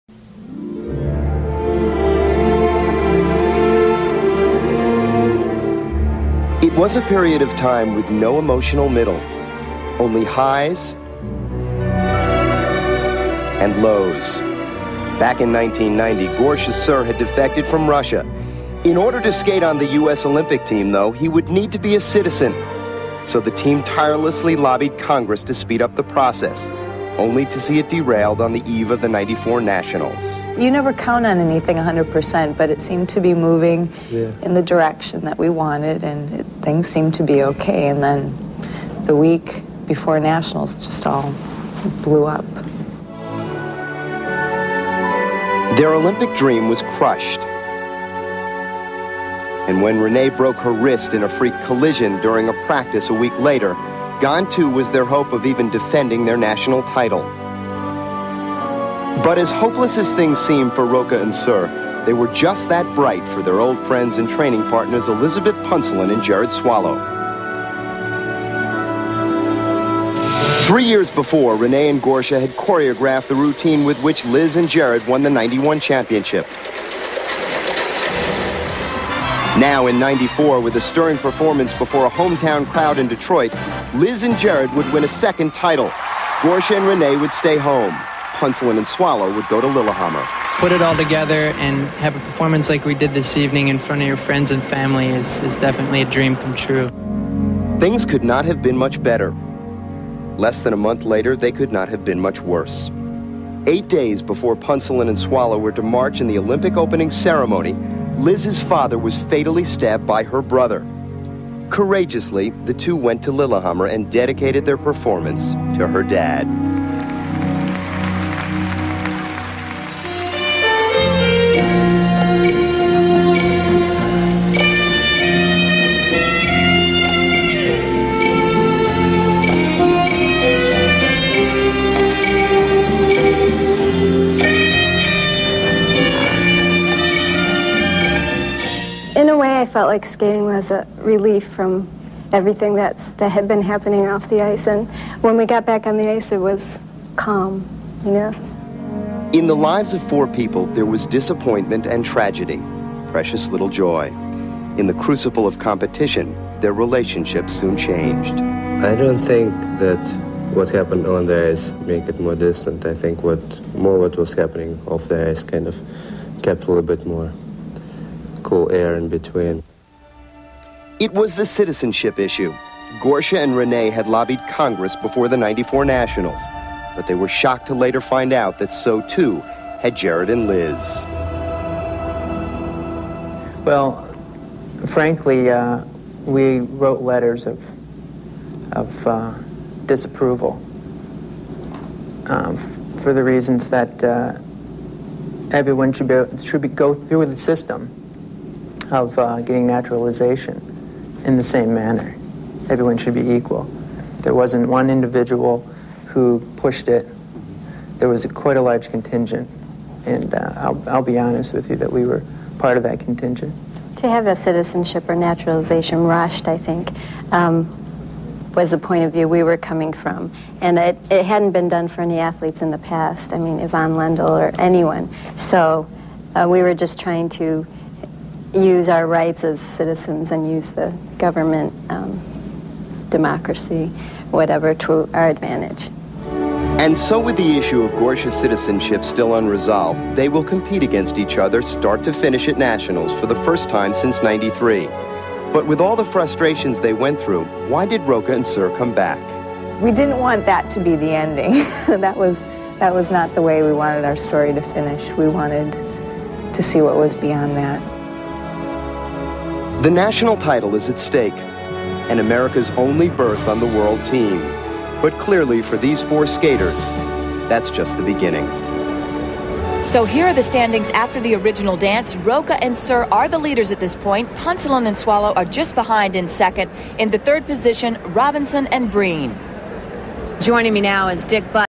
Piece on the Relationship between Roca & Sur and Punsalan & Swallow - 1995 U.S. Nationals
Providence, Rhode Island, USA - 1995 United States Figure Skating Championships, Ice Dancing, Free Dance - Piece on the Relationship over the last couple of years between Renée Roca & Gorsha Sur, who were once friends with and choreographers for Elizabeth Punsalan & Jerod Swallow, and Punsalan & Swallow.